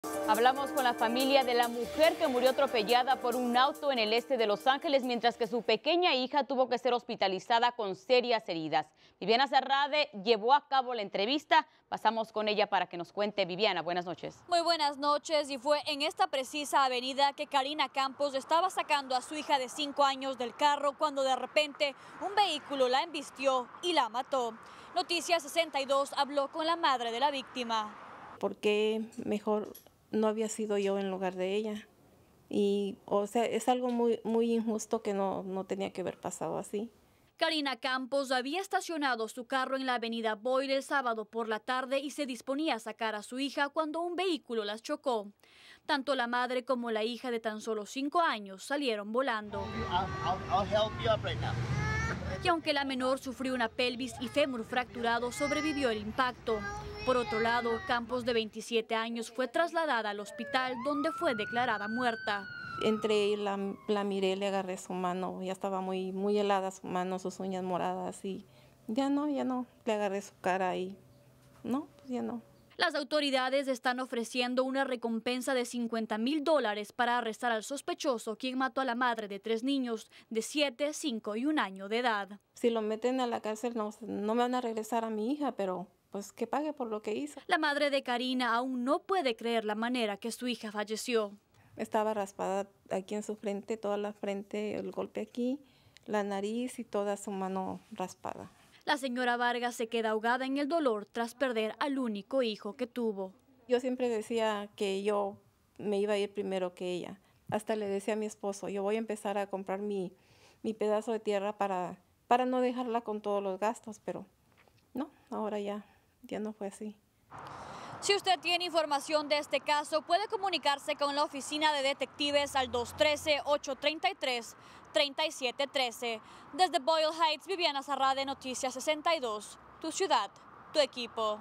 FAMILIA DE MUJER ATROPELLADA EN EL ESTE DE LOS ANGELES - Noticias 62
HABLAMOS CON LA FAMILIA DE LA MUJER QUE MURIO ATROPELLADA POR UN AUTO EN EL ESTE DE LOS ANGELES, MIENTRAS QUE SU PEQUEÑA HIJA TUVO QUE SER HOSPITALIZADA CON SERIAS HERIDAS.